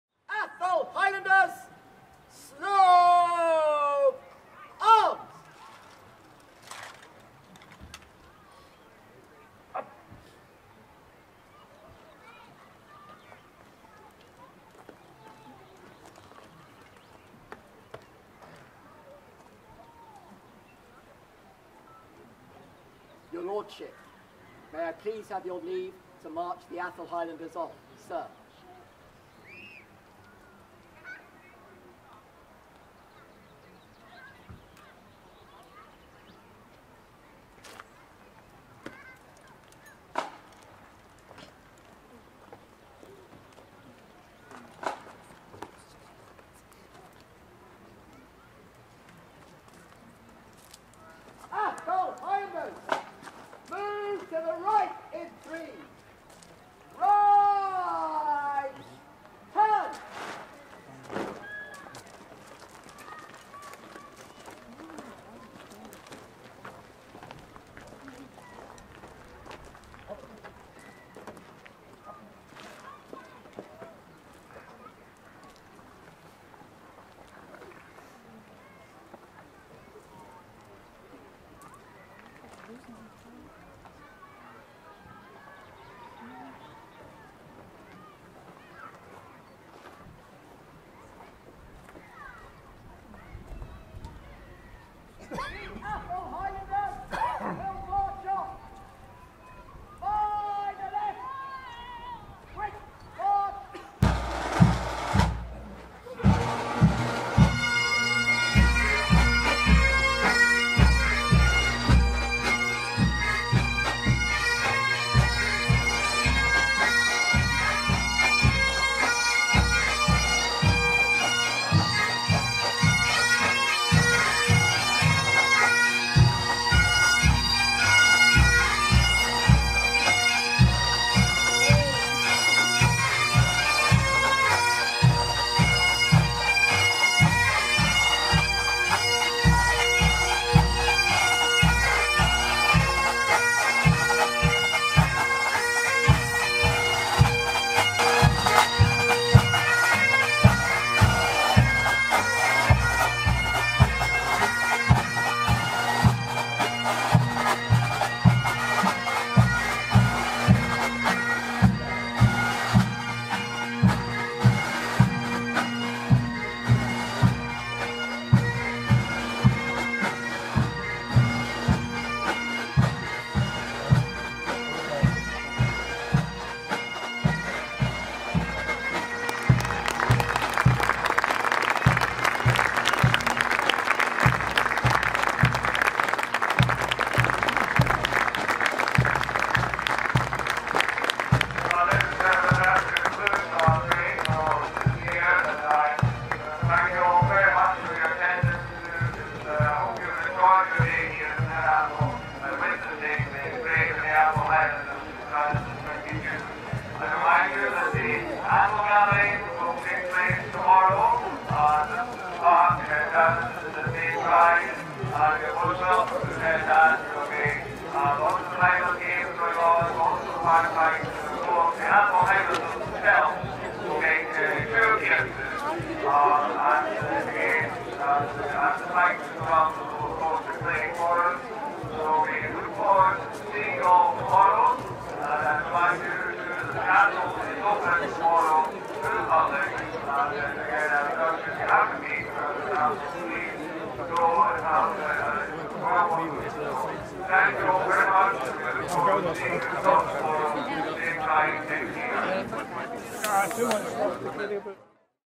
Atholl Highlanders 2022 - March Off
The annual parade of The Atholl Highlanders 2022 took place at Blair Castle, Blair Atholl on Saturday 28th May 2022.

This is the March Off